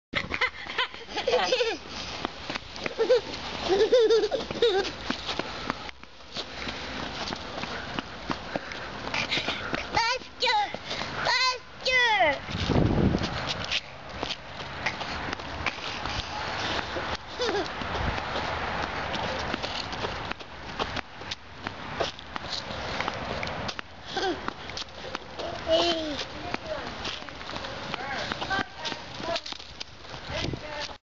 Emphatic Indigo Part 41 (Dizzycam)
Maggie chasing round and round in a circle - wanting to go "faster".